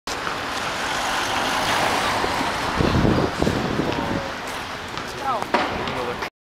サグラダファミリア
スペインのサグラダファミリアです。正面の様子を撮影しました。